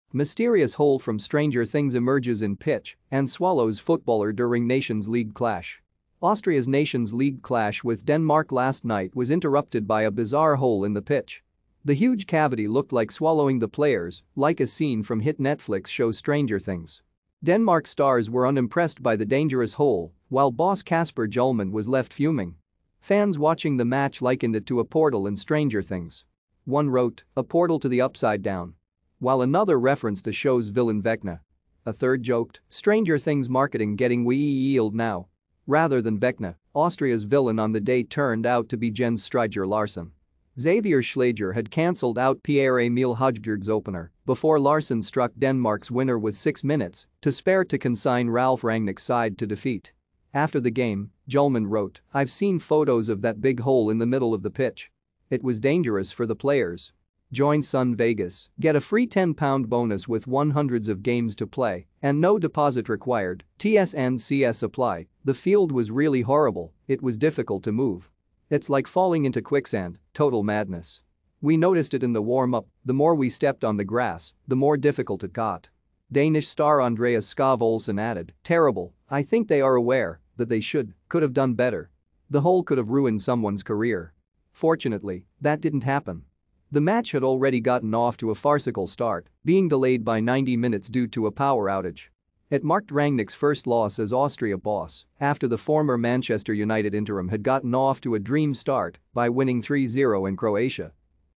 Mysterious hole ‘from Stranger Things’ emerges in pitch and SWALLOWS f
Mysterious hole ‘from Stranger Things’ emerges in pitch and SWALLOWS footballer during Nations League clash